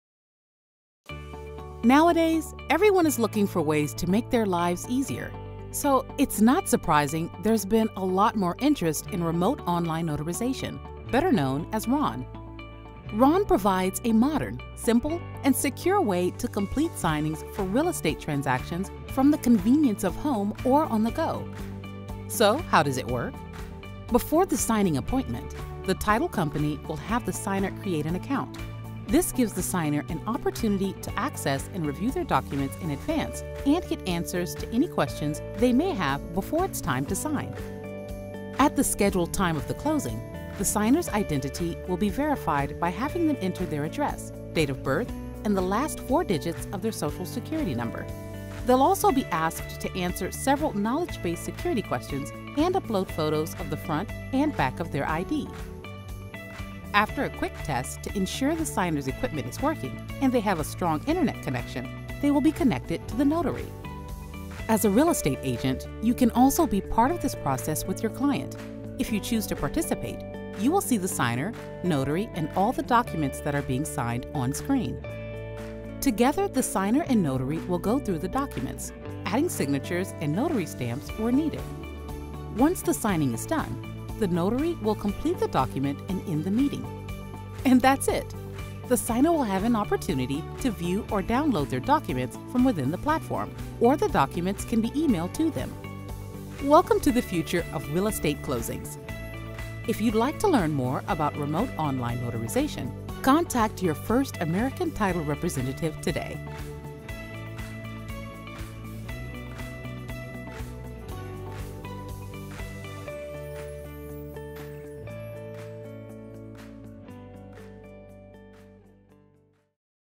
What is Remote Online Notarization (RON)?
Remote Online Notarization (RON) is a convenient, virtual way to sign your real estate closing documents. Watch our sample RON session to learn more.